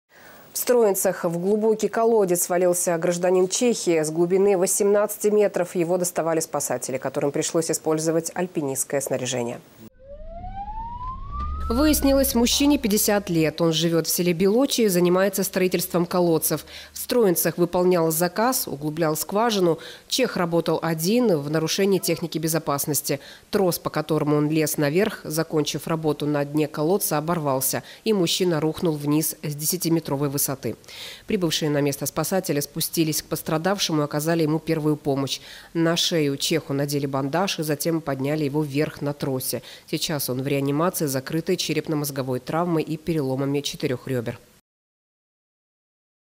Гражданин Чехии свалился в колодец в Строенцах
В Строенцах в глубокий колодец свалился гражданин Чехии. С глубины 18 метров его доставали спасатели, которым пришлось использовать альпинистское снаряжение.